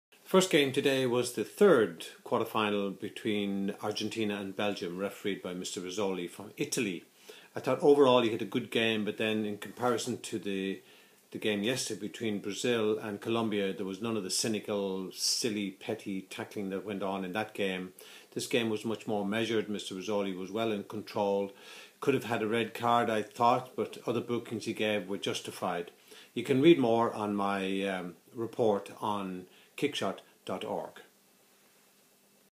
Argentina v Belgium Quarter Final WC2014 Referee Analysis
Third Quarter Final game World Cup 2014, Argentina v Belgium, Referee Analysis by "The Hanging Judge" Dr. Errol Sweeney, July 5, 2014; Referee Nicola Rizzoli - Italy; For a complete analysis,